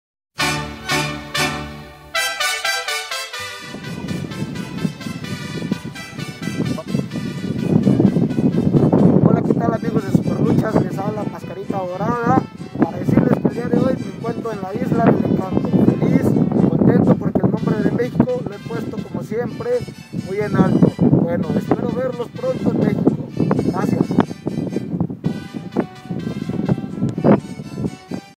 Mascarita Dorada envía Saludos a Súper Luchas desde Puerto Rico
Mascarita Dorada envía Saludos a Súper Luchas desde Puerto Rico desde el castillo del moro en San Juan.